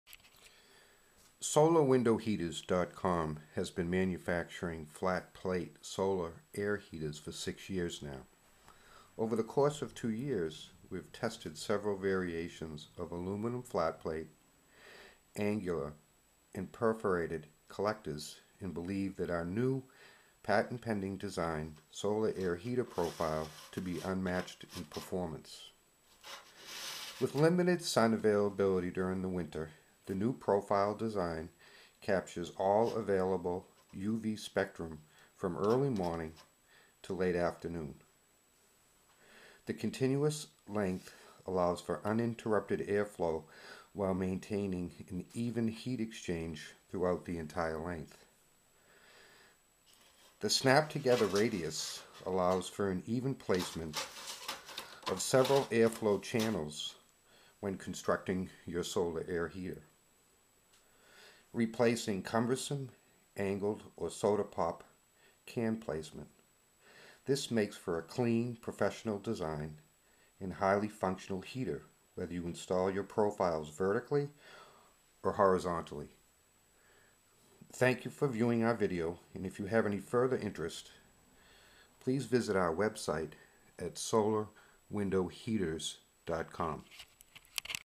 Solar Air Heater
Solar Air Heaters are a technology that have been around for years. It's always been a "do-it-yourself" chore to build one.

It's never been easy until now!

With our new solar air heater manifold kit along with several solar air heater profiles, you can build a collector in a few hours.